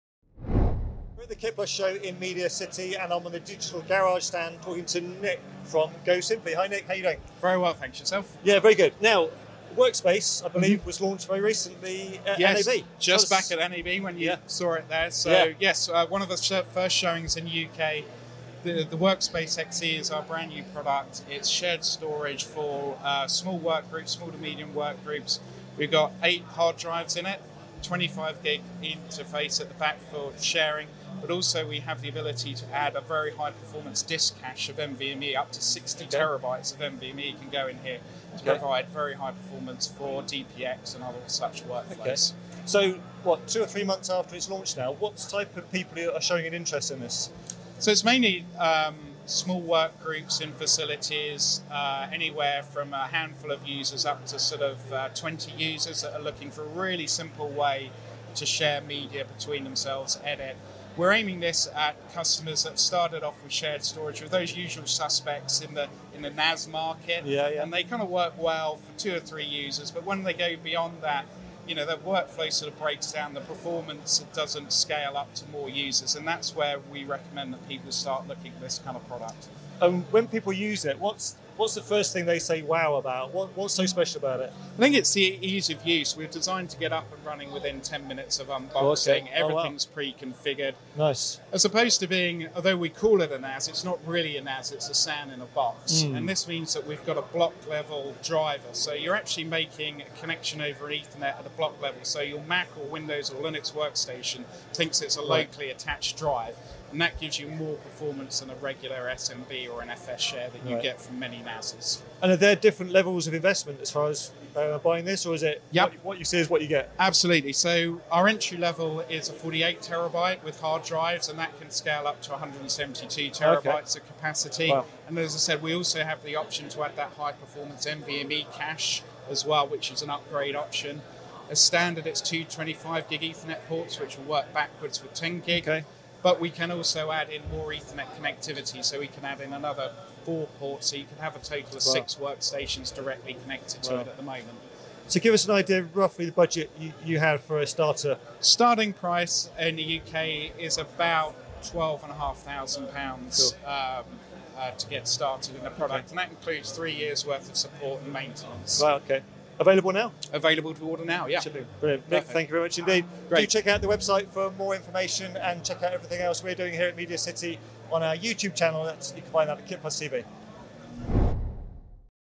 Symply WORKSPACE XE on Digital Garage Stand at KitPlus Show 2023
At KitPlus Show 2023 we speak to Nick from Symply about the newly launched WORKSPACE XE, on the Digital Garage stand, shared storage for small to medium work groups.

KitPlus Show is a regional one day show with exhibitors, workshops, training and seminar sessions. It's free to attend and you can check out the next KitPlus Shows at Glasgow and London later this year